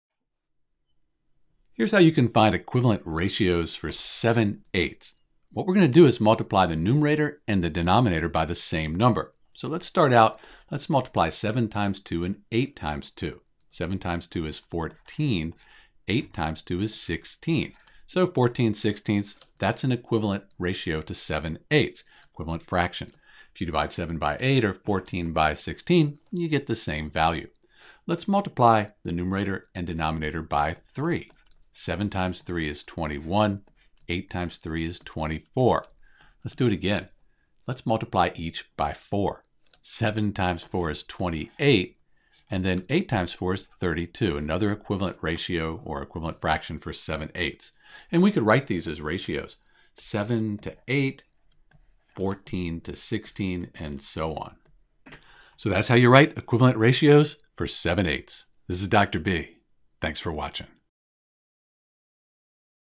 How to Write Equivalent Ratios for 7/8 (Seven-Eights)
To find three equivalent ratios for 7/8, we can multiply the numerator and denominator by the same factor. For example, if we multiply them by 2, we get 14/16, which is equivalent to 7/8. Dividing 7/8 or 14/16 gives us the same decimal value (0.875).

Note that the term equivalent ratio is sometimes called an equivalent fraction, although they do have slightly different definitions.

Another equivalent ratio for 7/8 can be obtained by multiplying the numerator and denominator by 3, which gives us 21/24.

Similarly, we can multiply 7/8 by 4/4 to get 28/32, which is yet another equivalent ratio for 7/8.

As long as we multiply the numerator and denominator by the same factor, we will get an equivalent ratio.